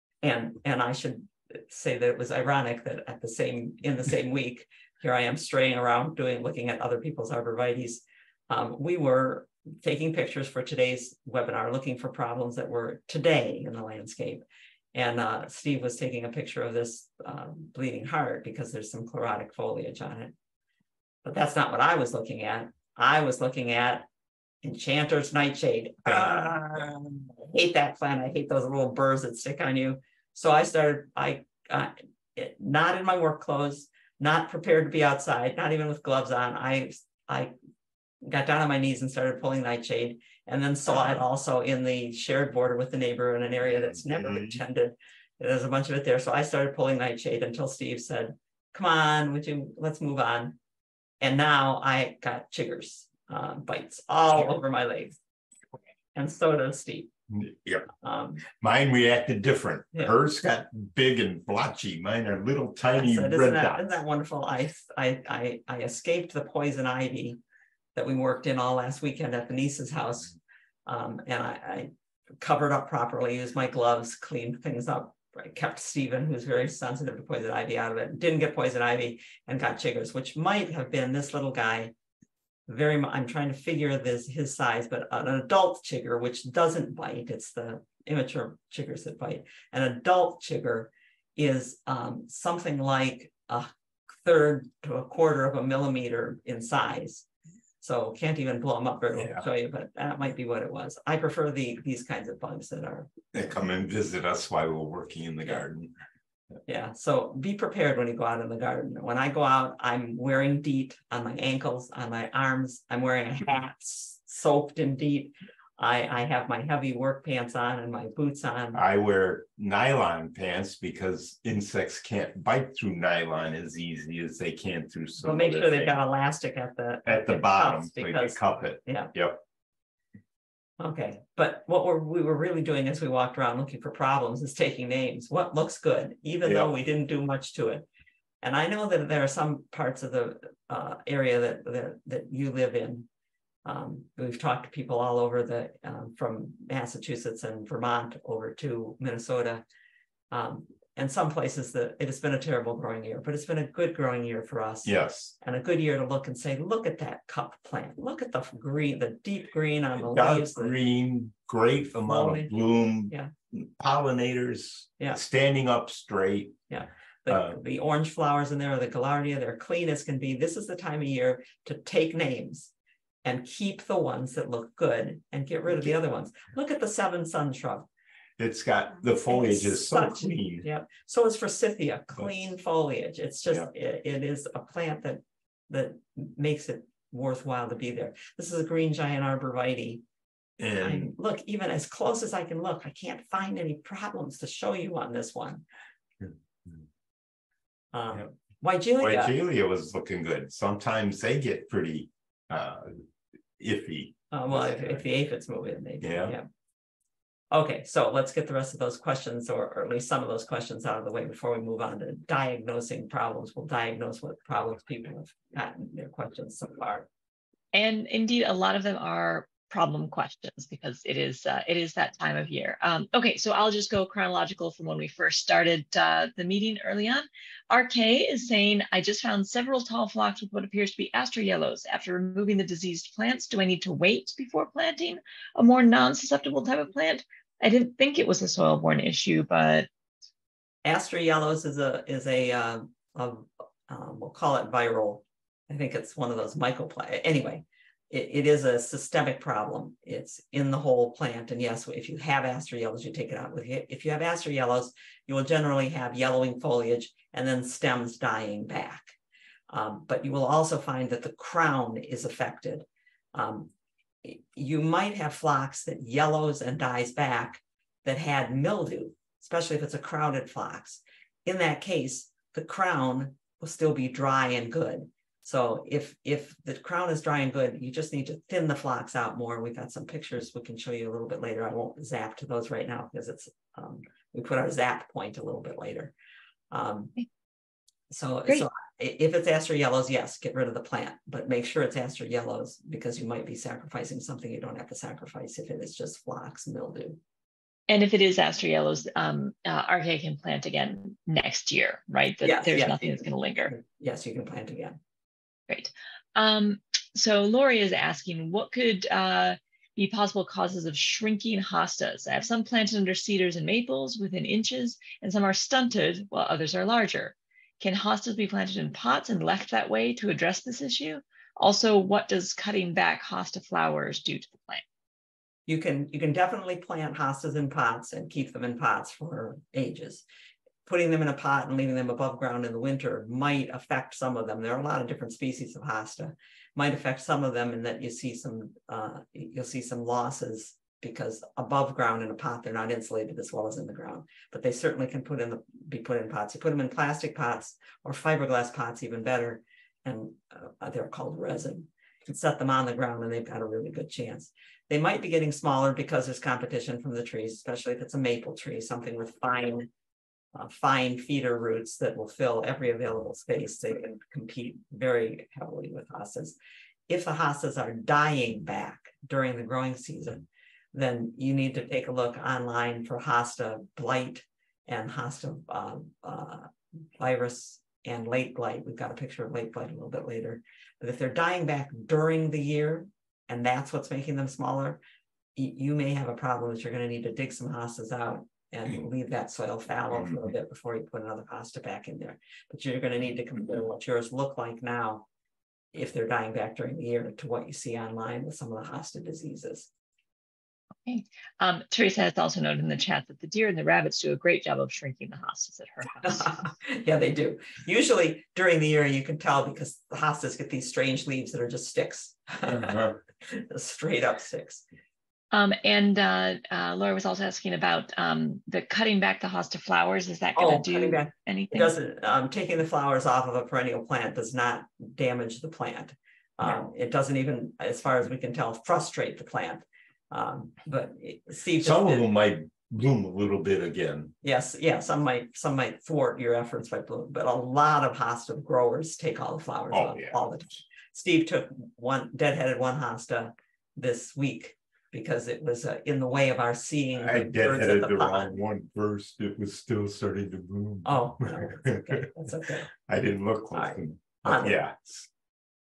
Weekend Walkabout 143 In Our Gardens Part 5
Regarding chippers, noting what looks good now, phlox with aster yellows, shrinking hosta fixes, license to deadhead hostas.